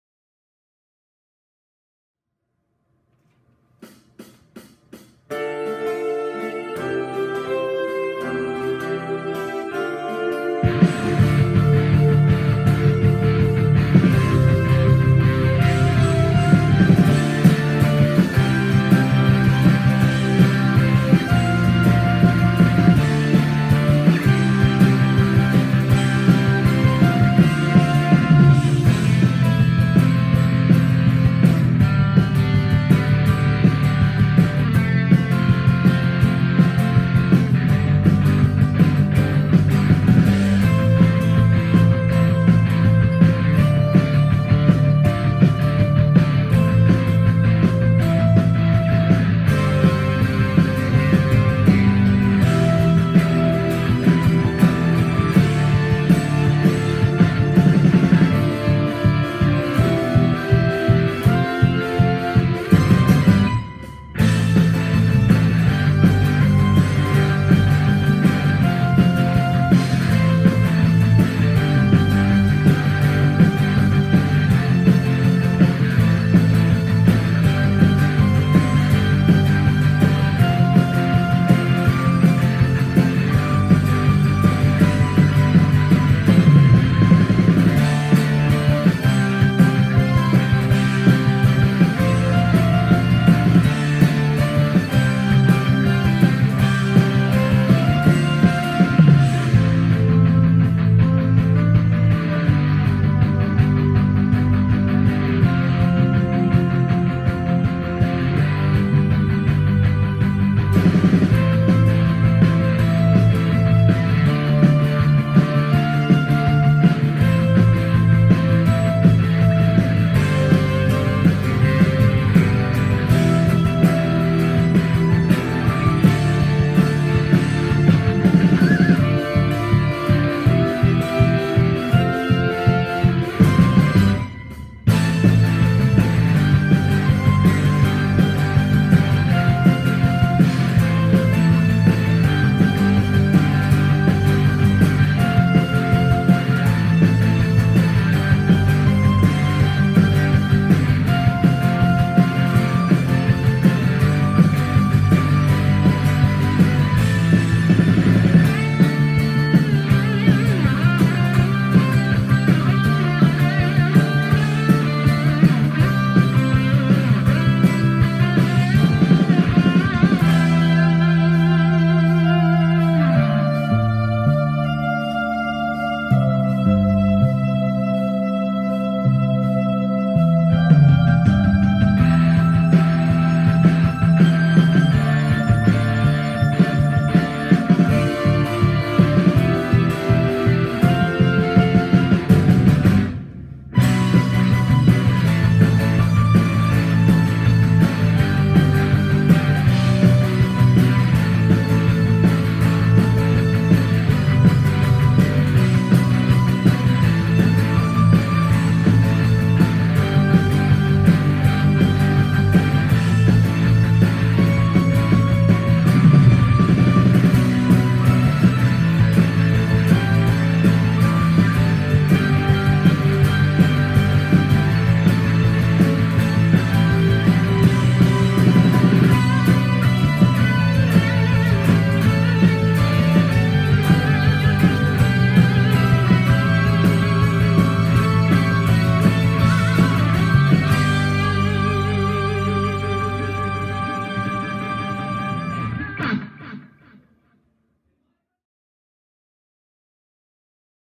広島コンピュータ専門学校音楽制作コース２年「３Dogs」2015
広島コンピュータ専門学校音楽制作コース２年のユニット「３Dogs」のデモ演奏。2015年８月。